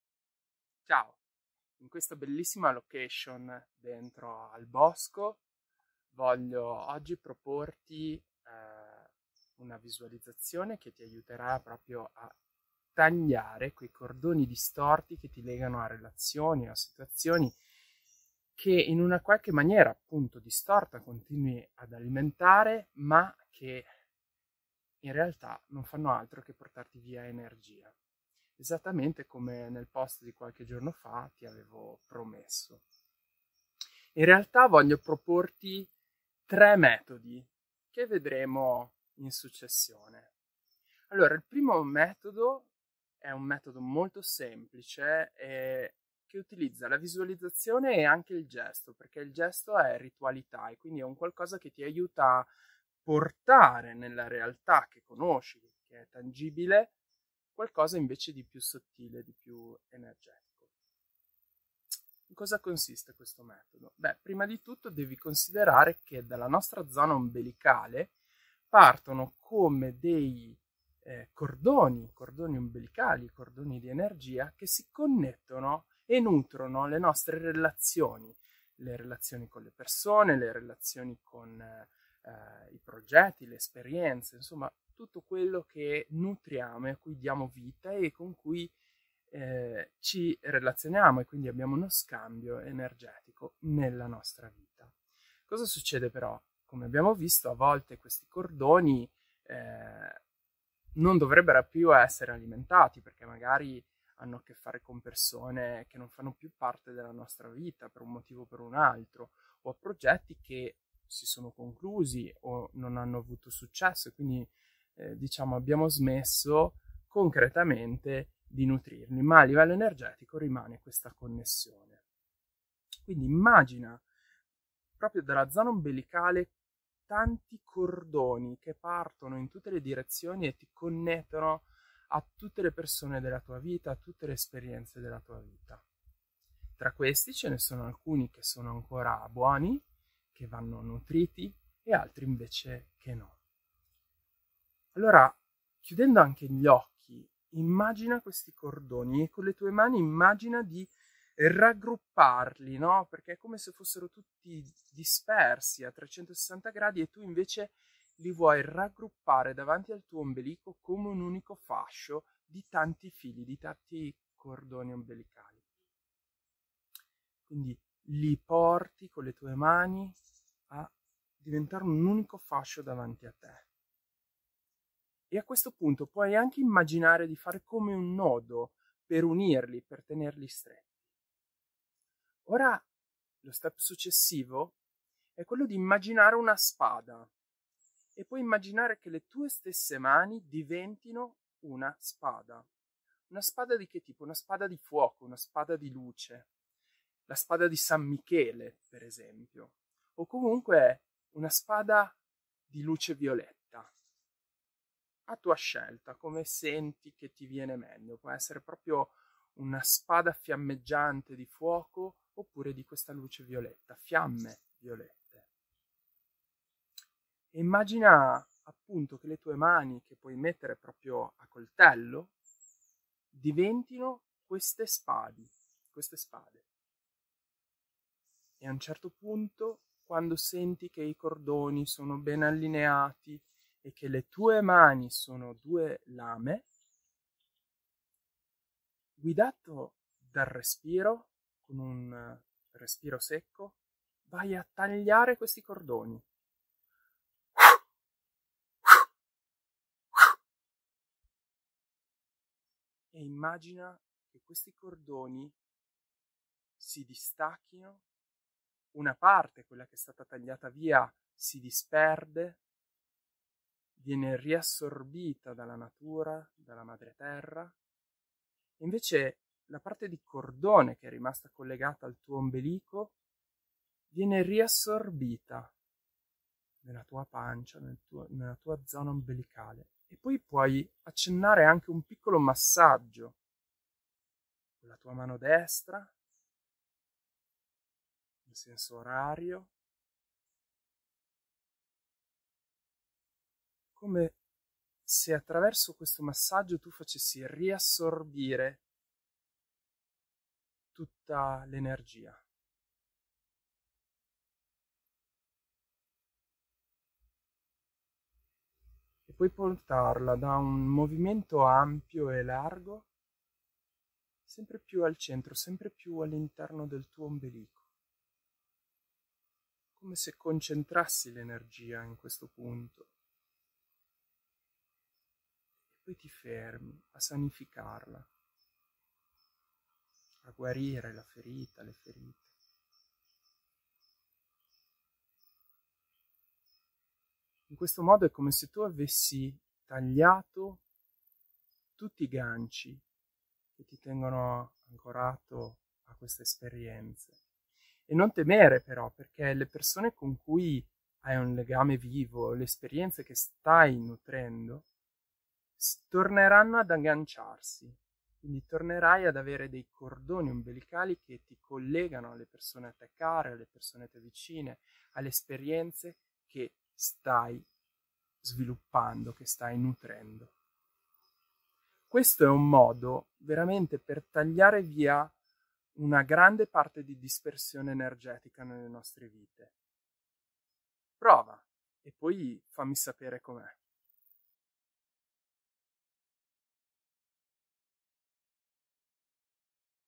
Come Liberarsi dalle Catene 1
Qualche giorno fa su Facebook ho parlato dei motivi per cui spesso ci sentiamo senza energia e non riusciamo a ricaricarci. Puoi leggerlo qui
Uno degli strumenti che ci può aiutare a stare meglio e a smettere di disperdere energia è tagliare 𝗶 𝗰𝗼𝗿𝗱𝗼𝗻𝗶 𝗲𝗻𝗲𝗿𝗴𝗲𝘁𝗶𝗰𝗶 𝗱𝗶𝘀𝘁𝗼𝗿𝘁𝗶 che non vuoi più alimentare.

Come promesso ho creato dei video in cui ti insegno delle semplici e veloci tecniche per tagliare questi cordoni e lasciare andare tutto ciò che non ti serve. Questo è il primo.

Ricorda: spesso per ottenere dei risultati non basta seguire una pratica una volta, ma bisogna allenarsi e ripeterla alcune volte... insomma allenarsi. In questo modo prenderai sempre più confidenza con la tecnica e potrai andare sempre più in profondità.  Quindi...
Sperimenta, sperimenta, sperimenta!
Valerio Folloni